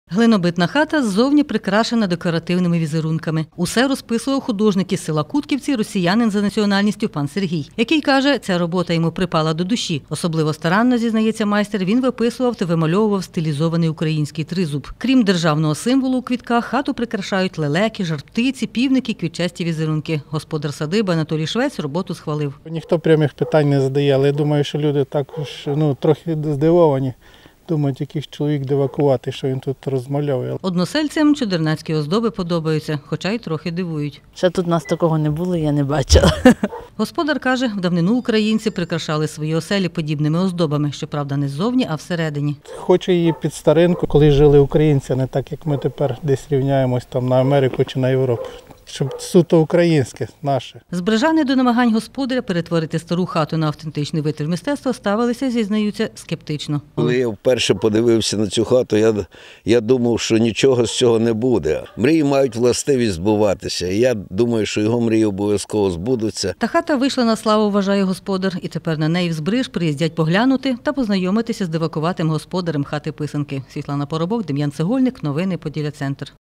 У хату «писанку» перетворилася глинобитна оселя у Збрижі Чемеровецького району
Хата-писанка існує у Чемеровецькому районі. Глинобитну оселю перетворив на витвір ужиткового мистецтва міський житель з Тернопільської області,котрий і придбав у Збрижі житло. Розписав його українськими візерунками та Національною символікою. Оселю в українському народному стилі бачила Світлана Поробок.